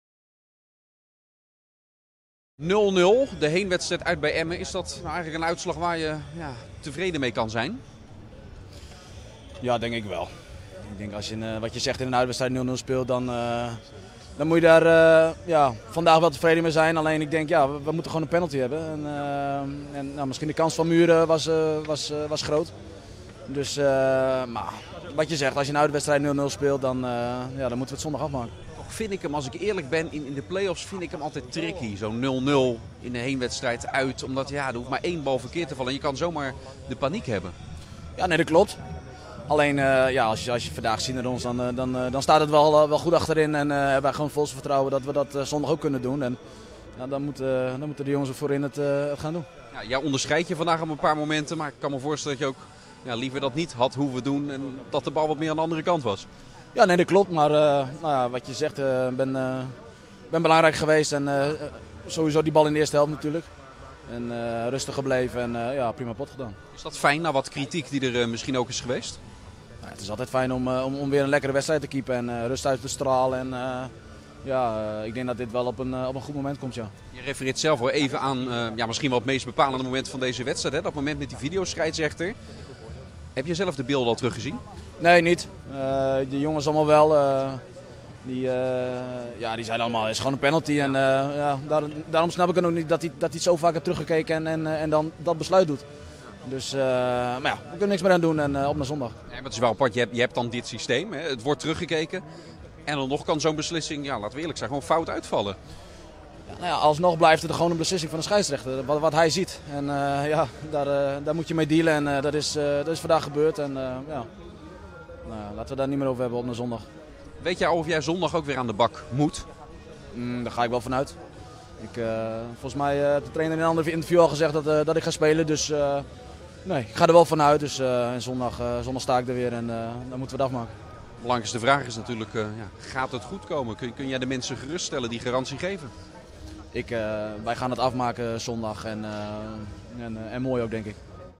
Leonard Nienhuis na FC Emmen-Sparta (0-0): 'Tevreden met uitslag'
De eerste confrontatie van de nacompetitiefinale tussen FC Emmen en Sparta is donderdagavond in Drenthe zonder winnaar geëindigd (0-0). Niet de uitgangspositie van de Rotterdammers, maar een opmerkelijke beslissing van de scheidsrechter op basis van videobeelden was na afloop van het duel het onderwerp van gesprek. In de beginfase kreeg Sparta twee aardige kansen. Robert Mühren was na acht minuten het eindstation van een mooie Rotterdamse aanval, maar de Volendammer stuitte op FC Emmen-keeper Dennis Telgenkamp. Twee minuten later ging een poging van Kenneth Dougall, die opereerde als centrale verdediger, net naast het Drentse doel.De grootste kans was echter voor de thuisploeg. Anco Jansen had een goede kopbal in huis, maar Leonard Nienhuis redde in de dertiende minuut op stijlvolle wijze. Vervolgens werkten FC Emmen en Sparta een povere eerste helft af, waarin de spanning vergoedde een hoop. Al scoorde Fred Friday een minuut voor de rust bijna de openingstreffer.Bizarre rol videoscheidsrechterMaar de Video Assistent Referee (kortweg VAR) speelde de hoofdrol in de tweede helft. Na een matige vrije trap van de Rotterdammers bleek Stijn Spierings in het strafschopgebied van FC Emmen overduidelijk te zijn vastgehouden door Alexander Bannink. Scheidsrechter Allard Lindhout liet de VAR naar dit moment kijken, maar tot ieders verbazing zag hij geen strafschop in deze fase.Na dit moment was de grootste kans voor Sparta. Friday hield in de 69e minuut goed het overzicht en zag Mühren helemaal vrij staan, maar vlak voor het doel van Telgenkamp knalde de huurling van Zulte Waregem de bal hard over. FC Emmen was beter, maar scoorde ook niet.Voor Sparta is het noodzaak om komende zondag op Het Kasteel te winnen van FC Emmen. Wanneer de Drenten scoren, dan moeten de Rotterdammers minstens één goal meer maken. Bij een gelijkspel met doelpunten of een nederlaag is de ploeg van trainer Dick Advocaat gedegradeerd.UPDATE: Lindhout: &apos;Goed gaan evalueren&apos;Lindhout gaf later bij FOX Sports toe dat hij fout zat. &quot;Ik was niet overtuigd van een strafschop op basis van de tv-beelden die ik zag&apos;&apos;, aldus de scheidsrechter. Toen hij na afloop vanuit een andere camerapositie de gewraakte situatie terugzag, wist Lindhout beter: &quot;Het was een strafschop. Dit is niet positief, we moeten dit heel goed gaan evalueren.&apos;&apos;FC Emmen - Sparta 0-0 (0-0)Opstelling Sparta: Nienhuis; Holst, Fischer, Dougall, Floranus; Duarte, Mühren, Sanusi; Verhaar (73&apos; Dos Santos), Friday, Spierings (82&apos; Van Moorsel)